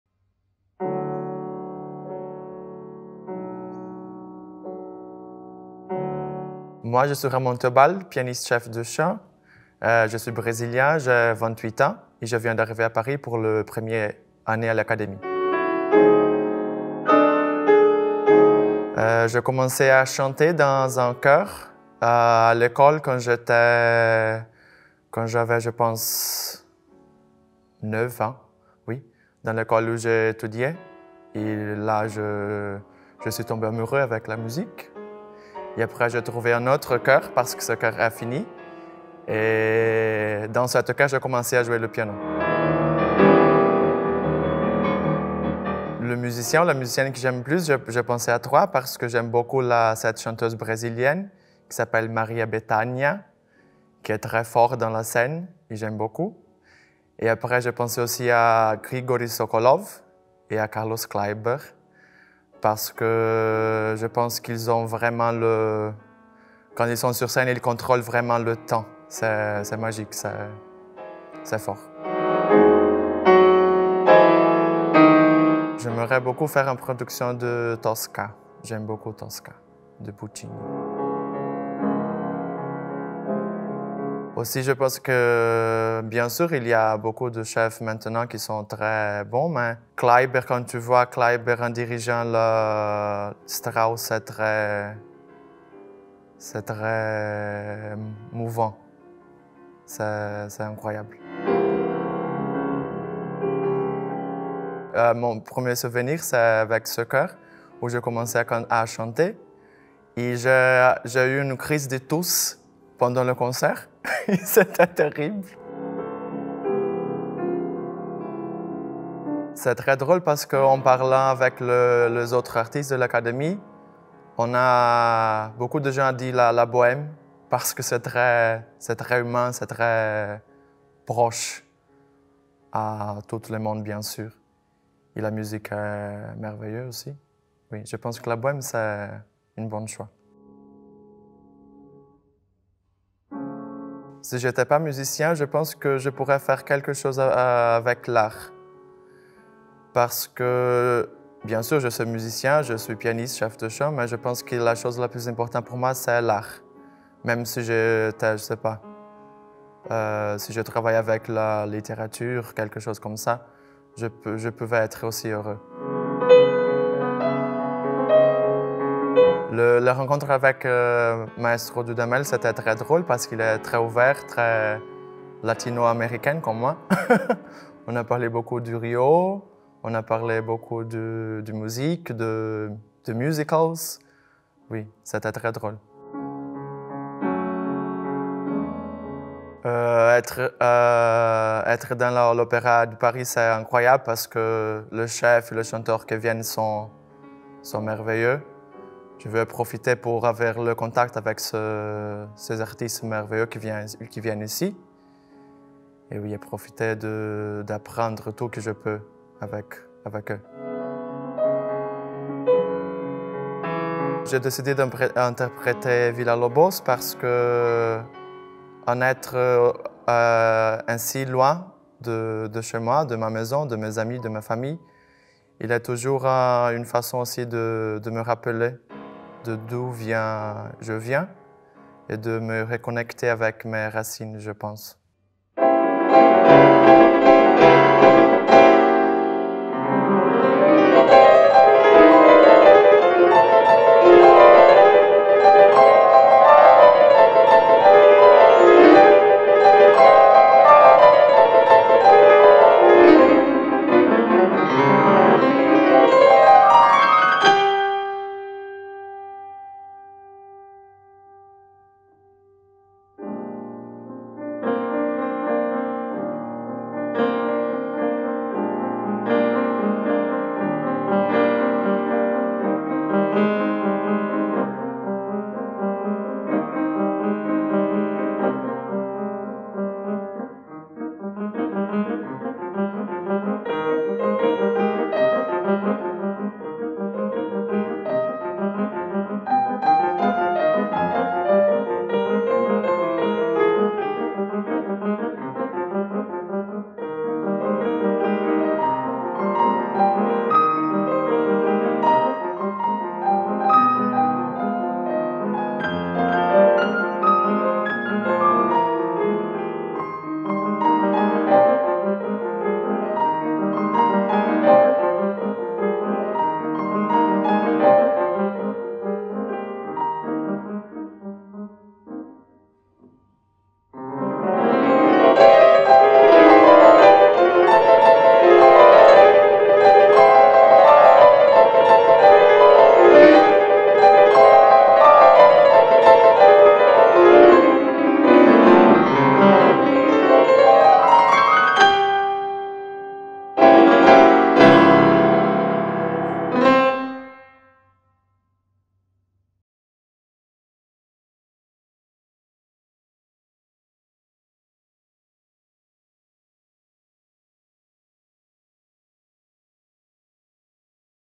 Journal d'un musicien #33 : RAMON THEOBALD
Découvrez Ramon Theobald, pianiste - chef de chant en résidence à l'Académie de l'Opéra national de Paris.